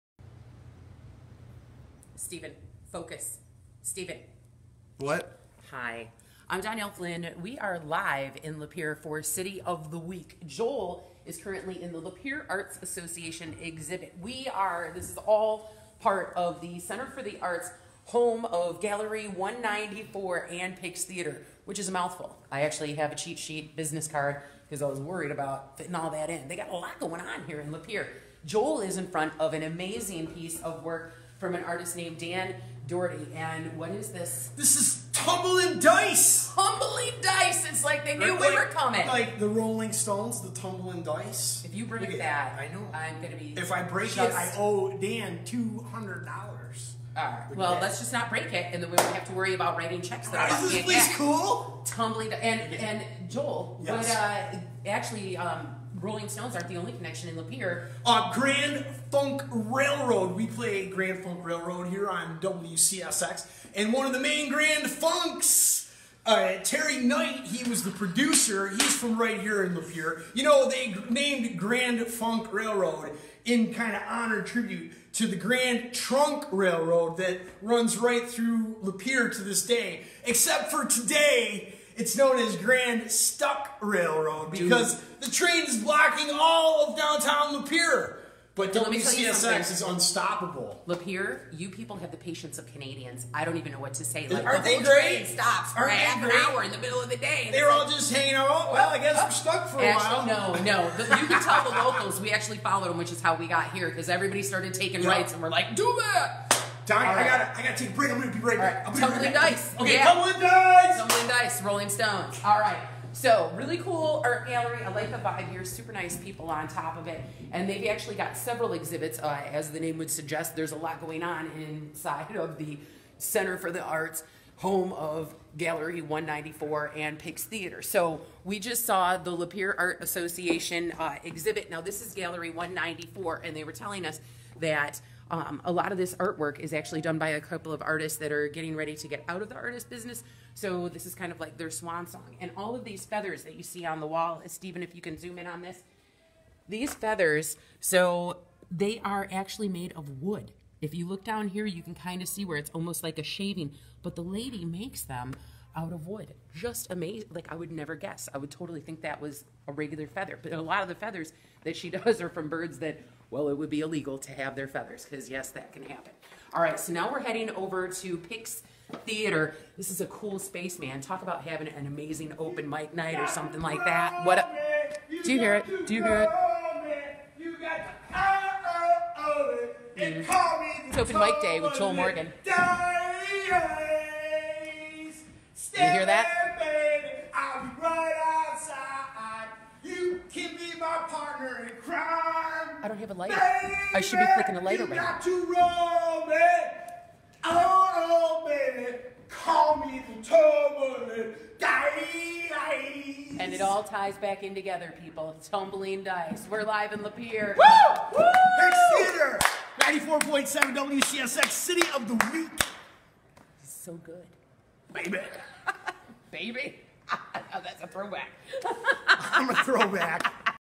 City of the Week: Lapeer, Jan. 17th, 2019
Grand Stuck Railroad! City of the Week live in Lapeer for an overview of cultural flavor. They're trying to class themselves up... good luck!